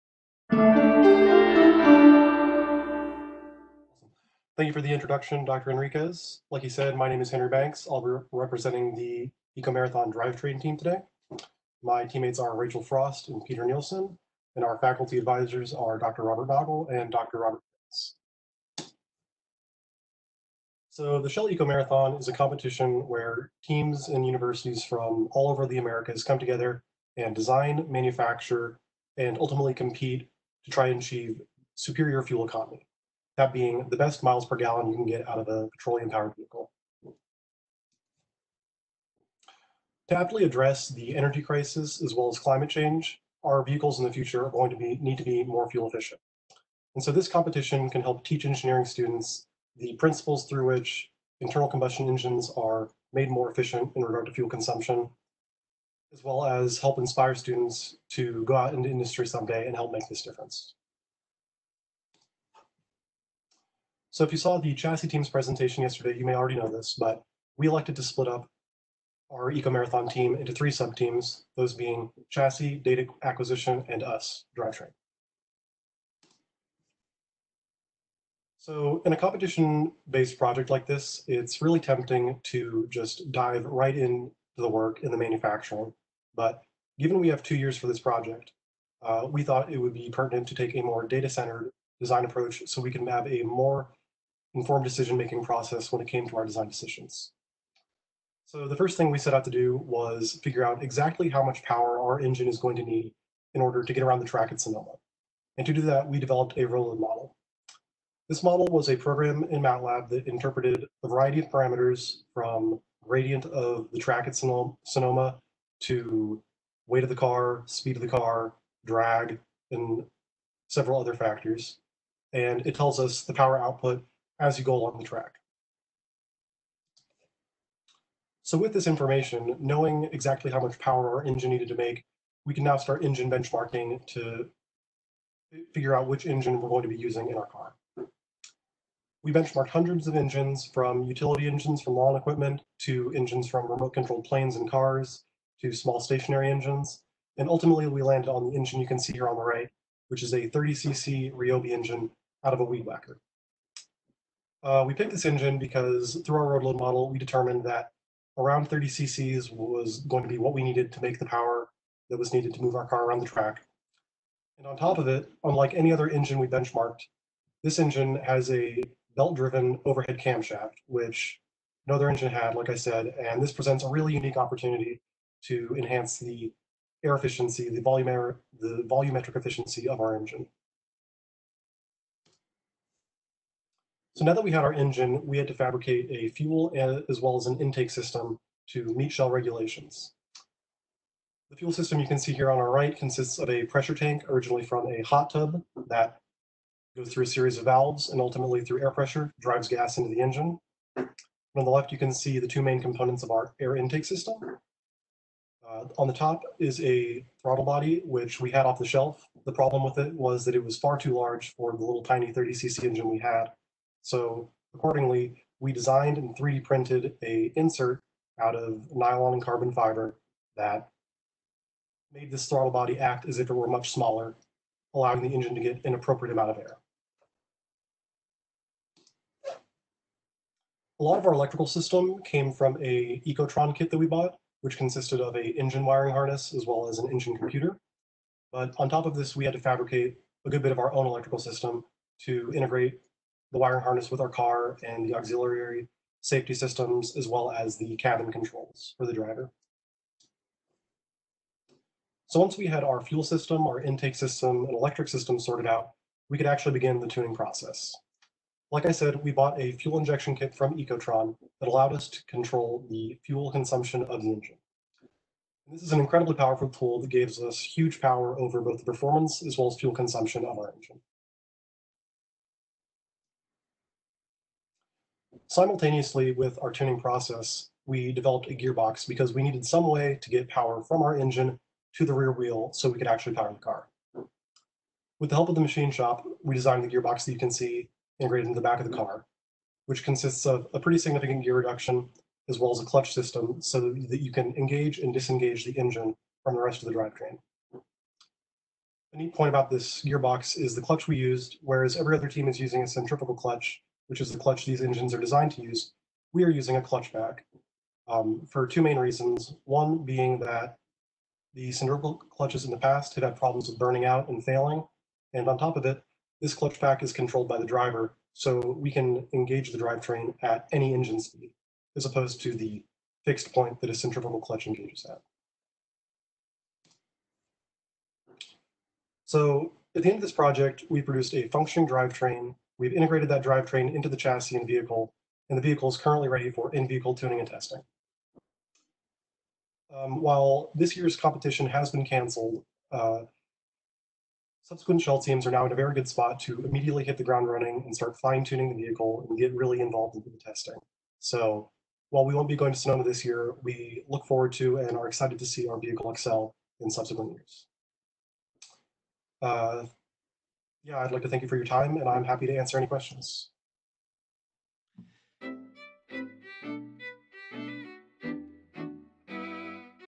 Shell Eco-Marathon - Drivetrain
This video is a student presentation from the 7th Annual Madison Engineering xChange (April 20-24, 2020, all-online due to Covid-19 pandemic) highlighting work during Year 2 of the two-year-long capstone project: Shell Eco-Marathon - Drivetrain